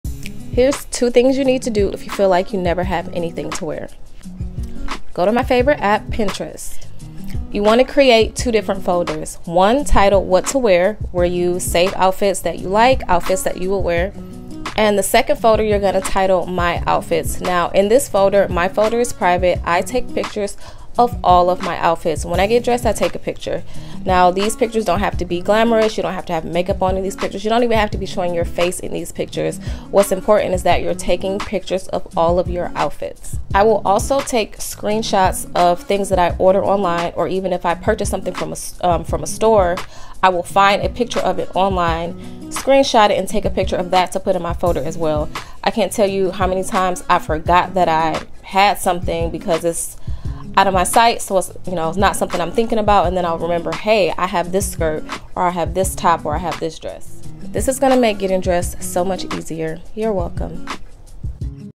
Nothing To Wear!? Try This!
I love Pinterest!! I spend hours on there looking for outfit ideas! It’s so much fun and makes getting dressed so much easier!!

Follow me for more fashion/amazon/TikTok shop/ beauty videos!